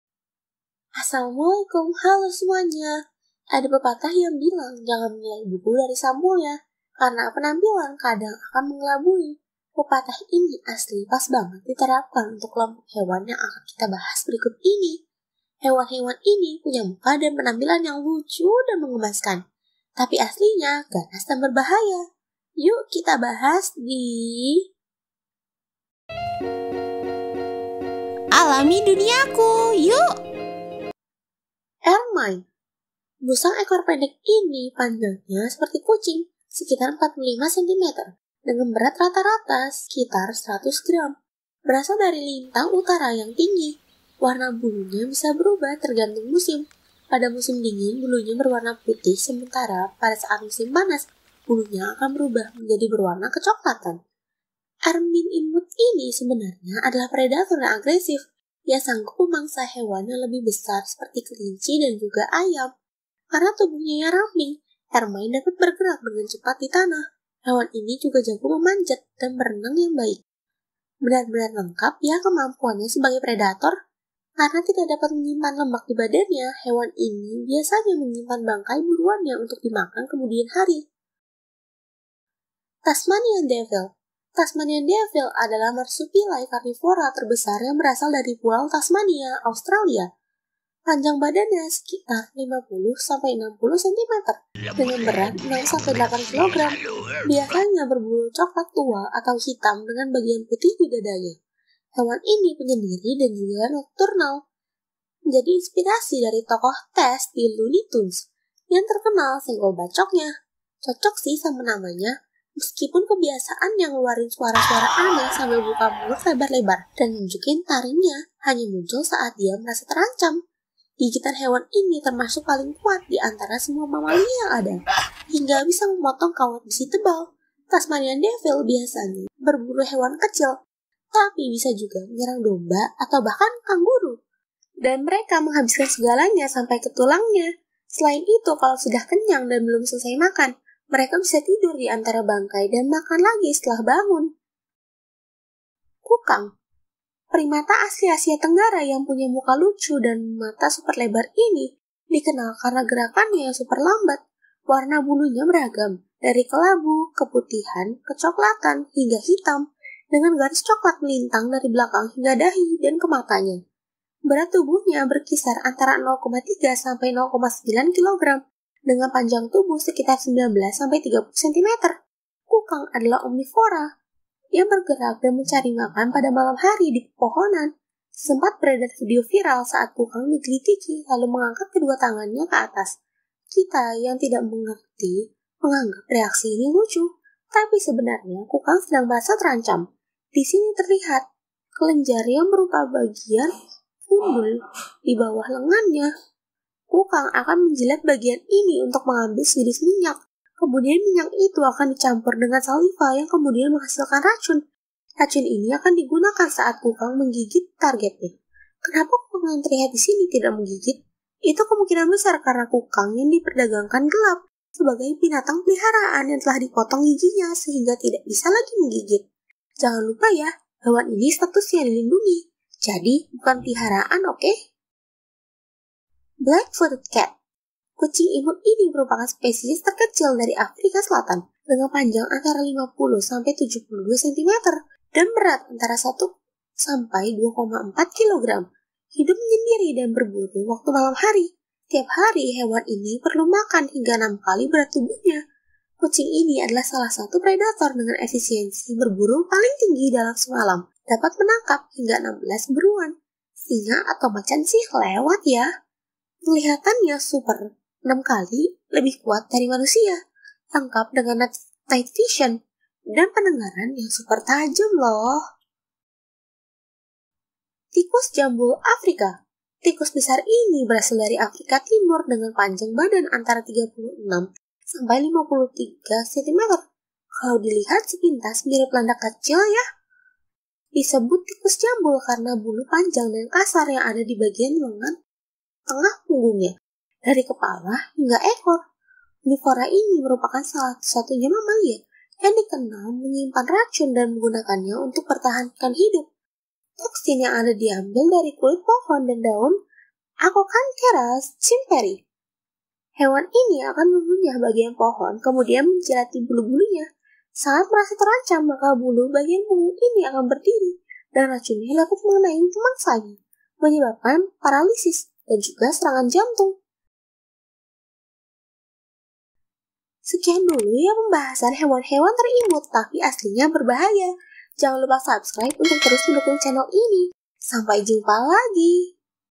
Hewan-hewan yang imut tapi aslinya ganas dan berbahaya
Kali ini kita bahas hewan-hewan yang imut dan menggemaskan tapi aslinya ganas dan berbahaya. Dari ermine, black footed cat dan tasmanian devil yang merupakan predator ganas, sampai kukang dan tikus jambul Afrika yang beracun.